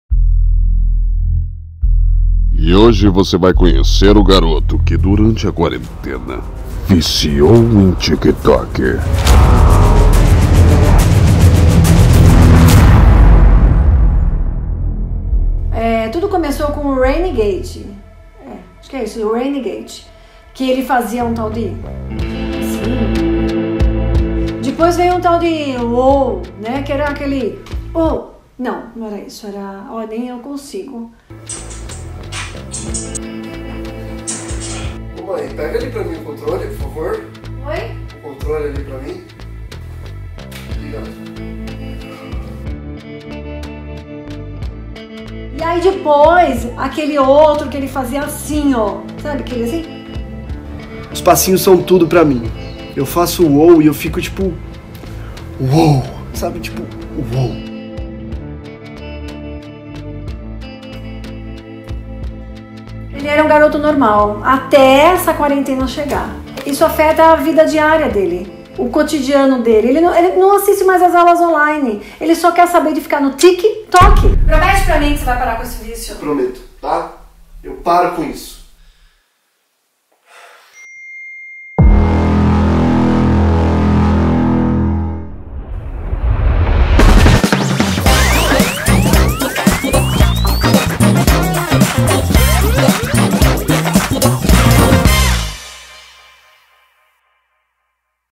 o VICIADO em Tik Tok - Pkllipe
Eu ja estava viciado no Tik Tok, mas agora com essa quarentena as coisas pioraram e o meu vicío vem se tornando real!

►Compartilhe com o seu amigo viciado!